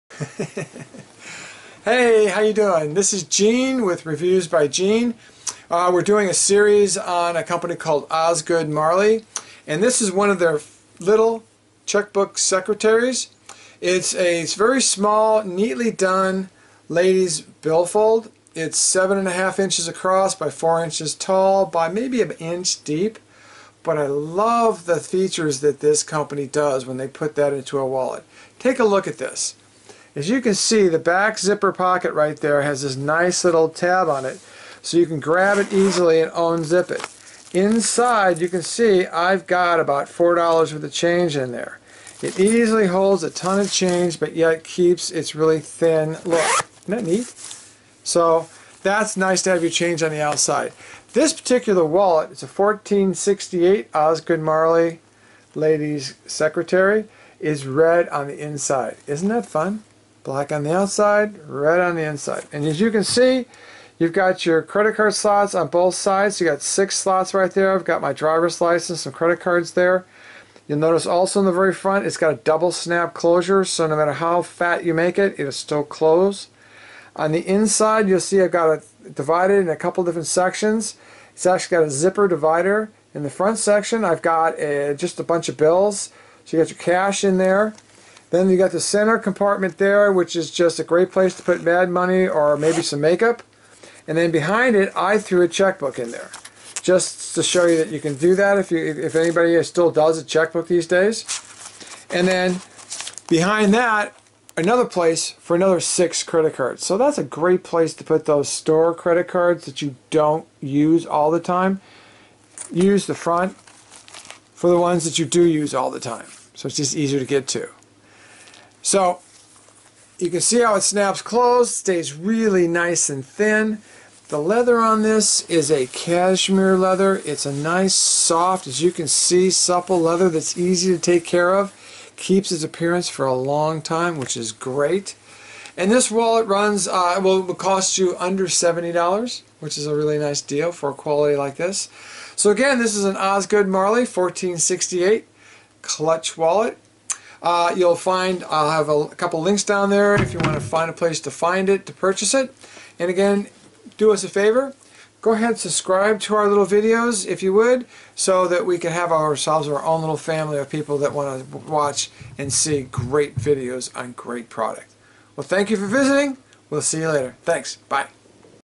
Osgoode Marley Premium Cashmere Leather Women's Checkbook Wallet
reviews the Osgoode Marley Cashmere Leather Women's Checkbook Wallet. This is one of the best women's checkbooks available and is 7.25" x 4" x 1". Inside is a zippered pocket, 6 card slots, gusseted pocket for bills, back coin pocket. Colors: Black W/red interior.